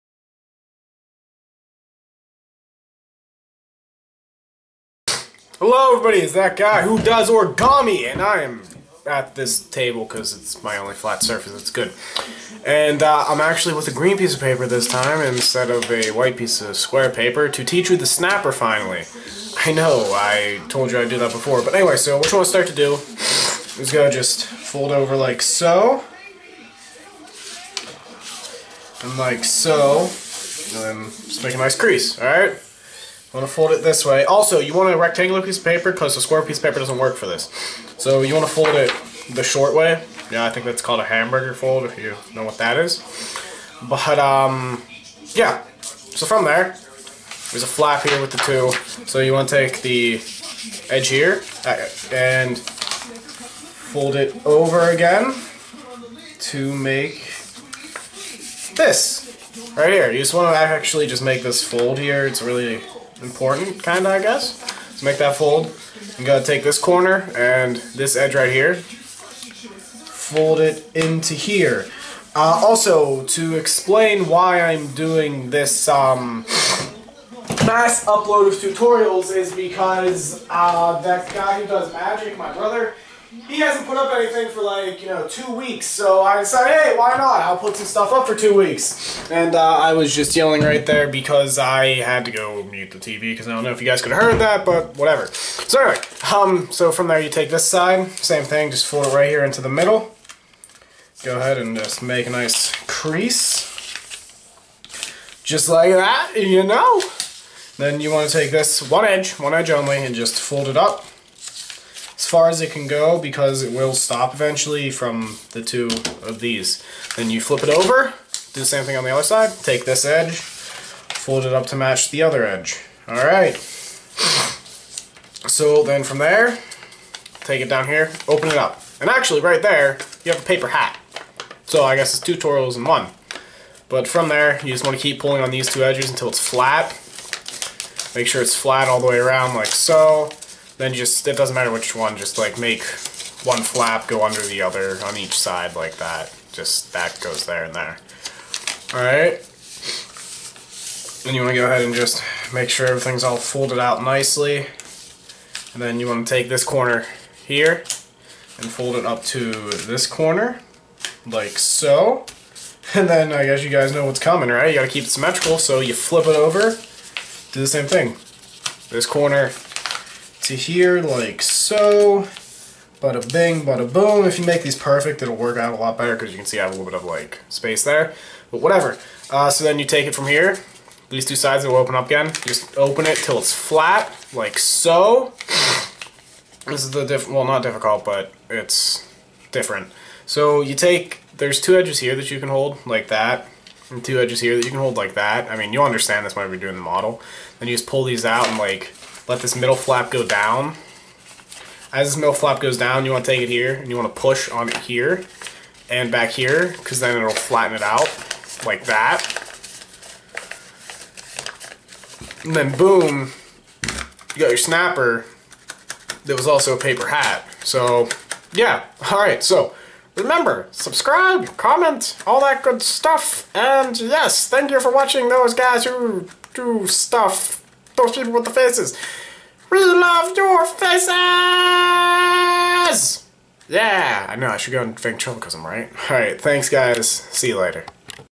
Origami How To SNAPPER
This is how you make a snapper, and something else, out of paper.  Its awesome watch it you wont be sorry :)

Also not my model i dont know who created it but i promise it wasnt me

FaceBook: Those People With The Faces